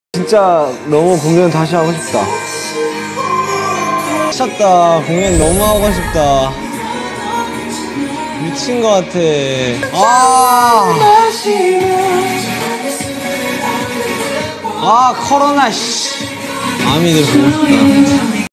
Baby Koo miss us ARMY'S and cries -he want to keep performing for Us | Jungkook crying | BTS
he love us ARMY'S so don't u ever think there is no one who loves u remember there is JK from BTS always loving us and not only JK but the whole BTS loves us ARMY'S so so much and they work hard for us💜